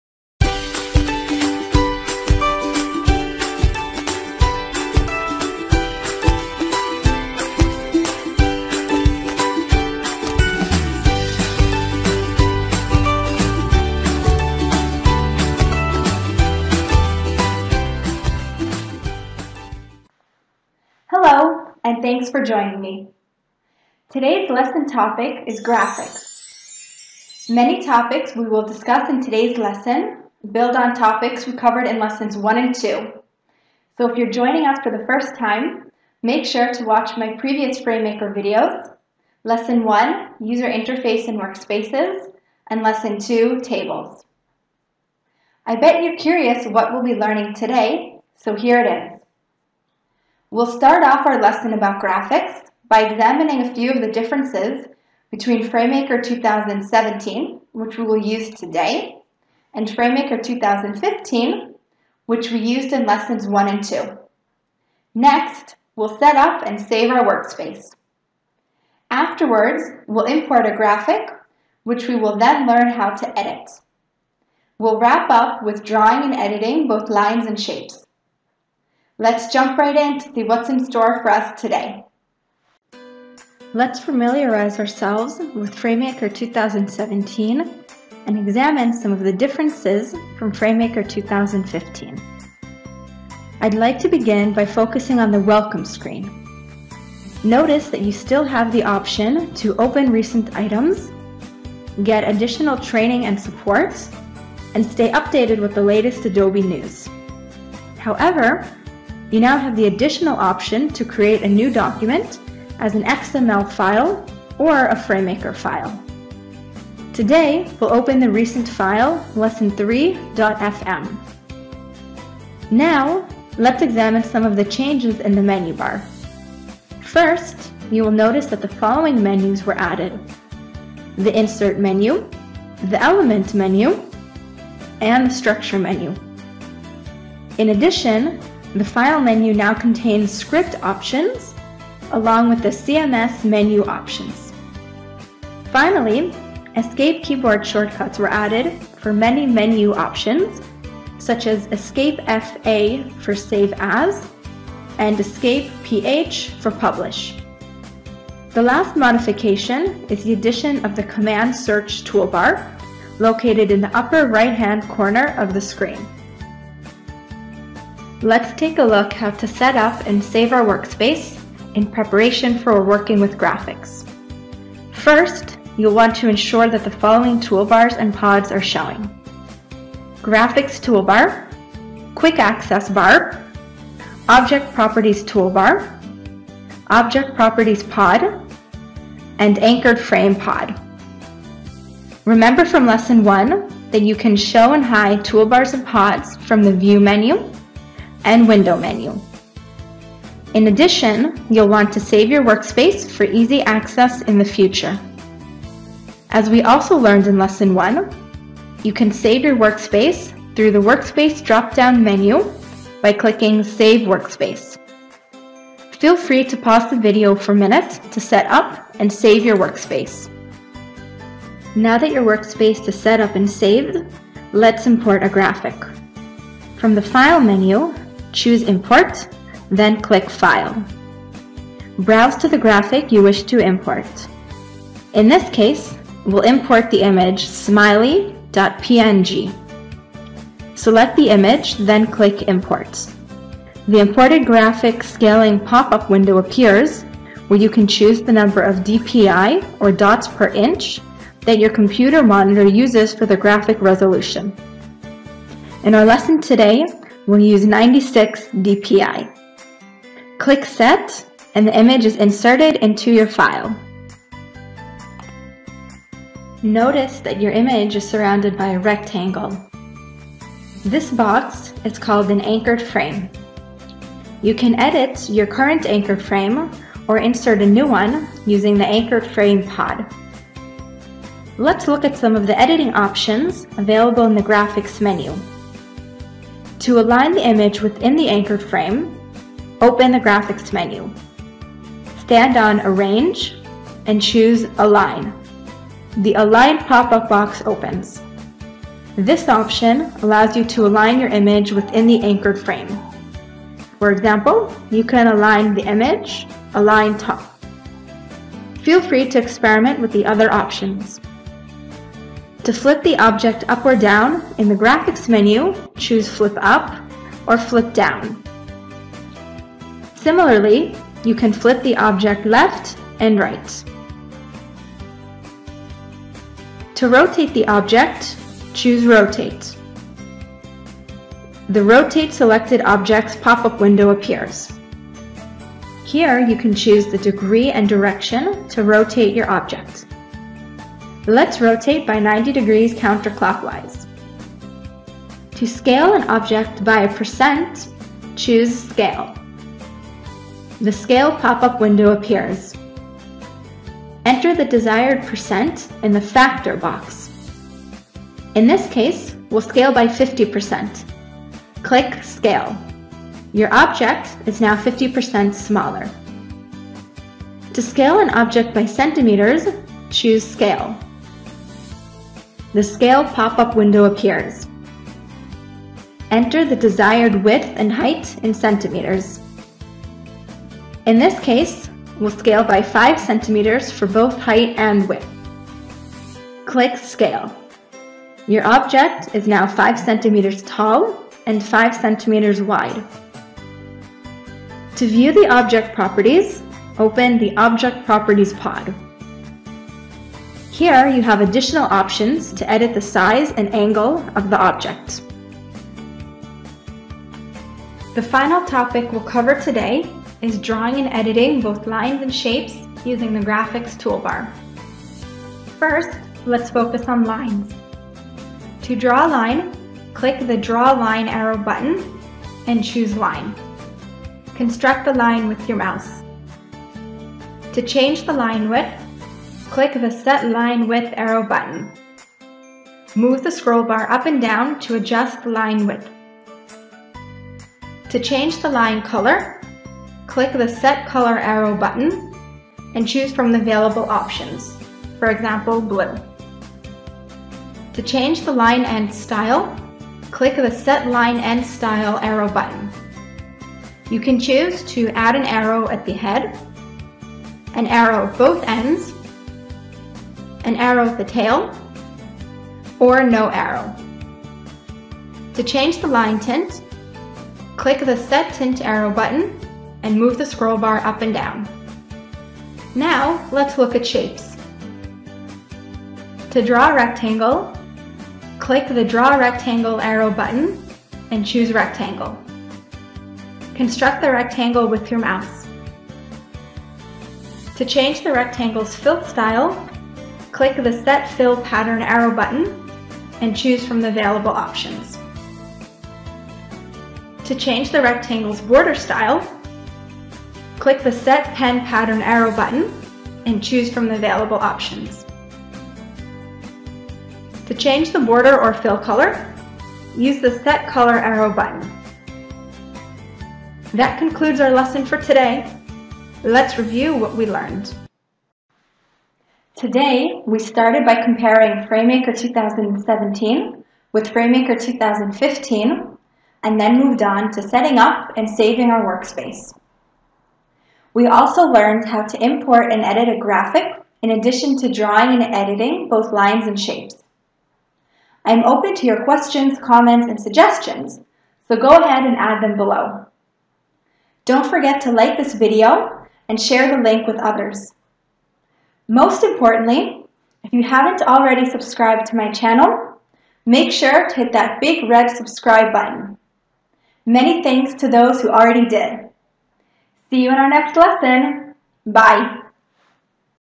FrameMaker Lesson 3: Graphics
Lesson 3 builds on our previous FrameMaker videos: Lesson 1: User Interface & Workspaces and Lesson 2: Tables

In Lesson 3, we briefly review some of the differences between FrameMaker 2017 and FrameMaker 2015, and focus on inserting and working with graphics.

I am looking forward to your questions, comments, and suggestions.